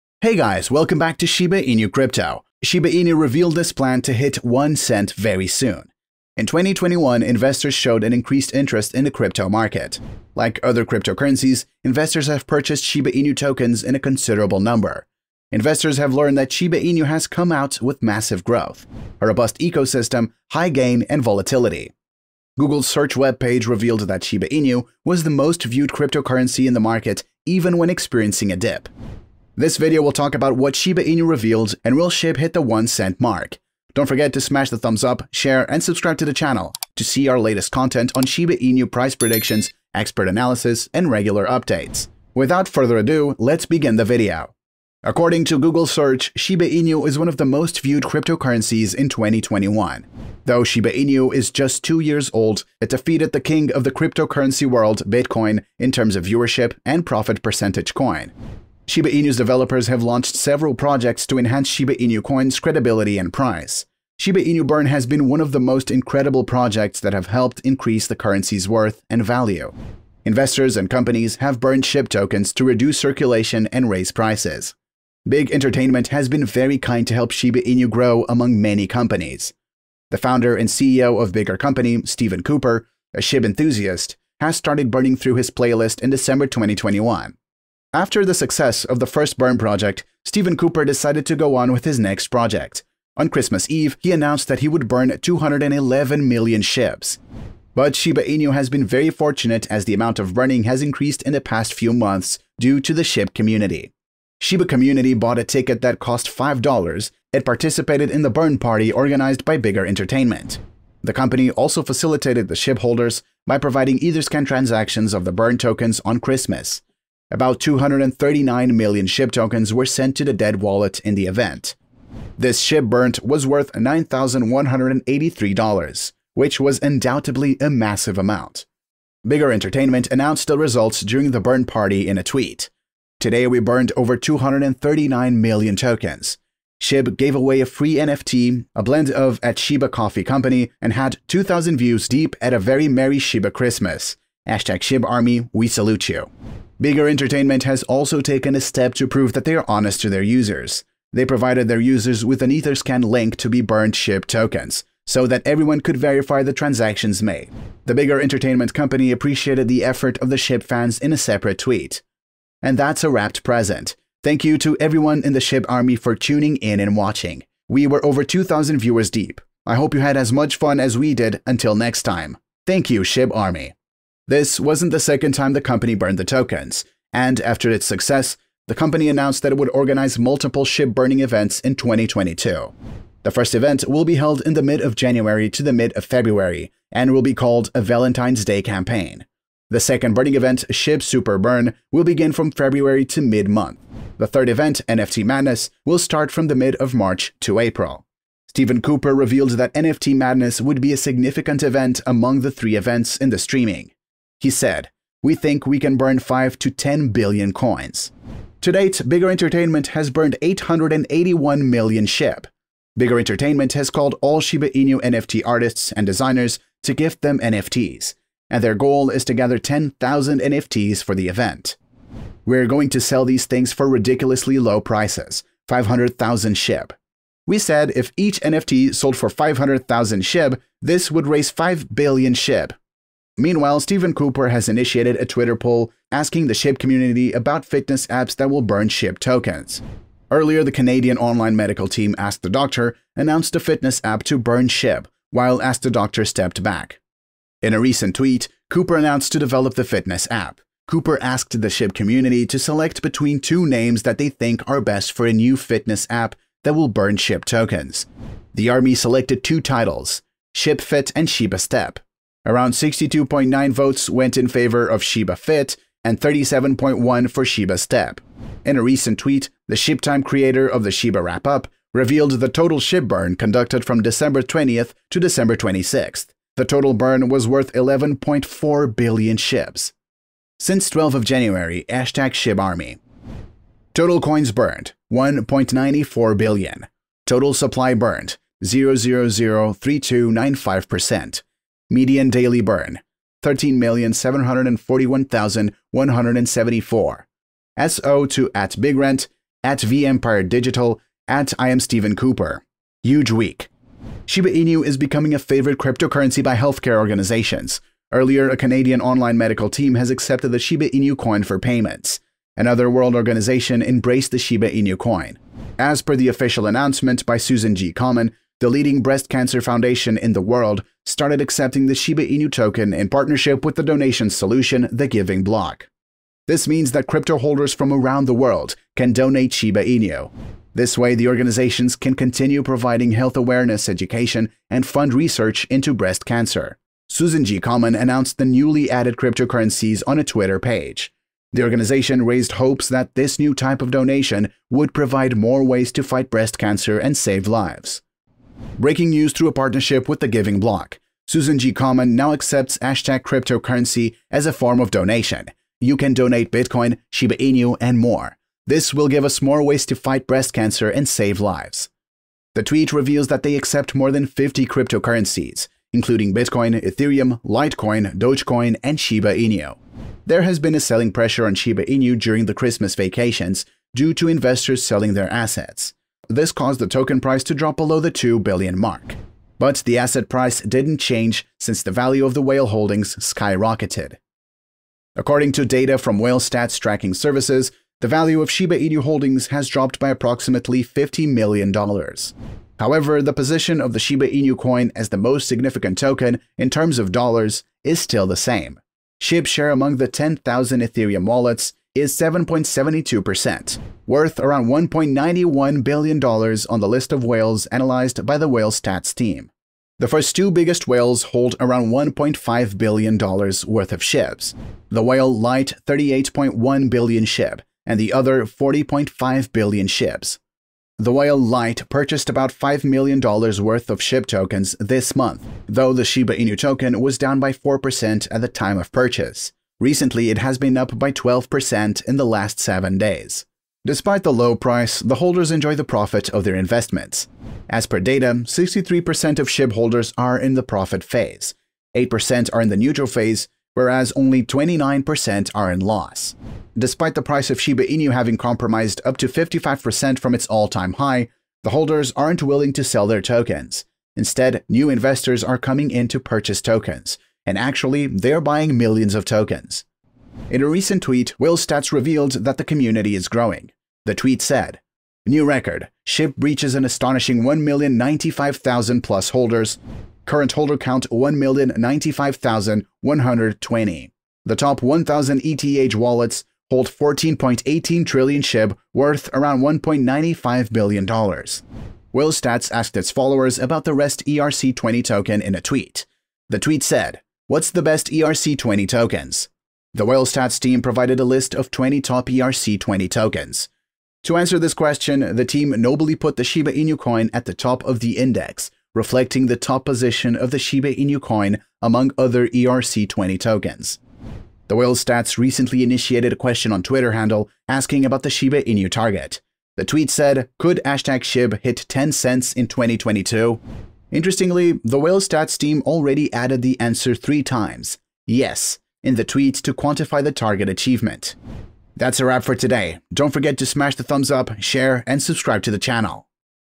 SHIBA INU REVEALED PLAN TO HIT $0.1 SOON! - Explained
Shiba Inu, Shiba Inu Coin News and Crypto News.

Today in this video we’re gonna talk about Shiba Inu who REVEALED this plan to hit $0.1 SOON!! The crypto market is always surprising us so we need to keep up to date with the cryptocurrency news.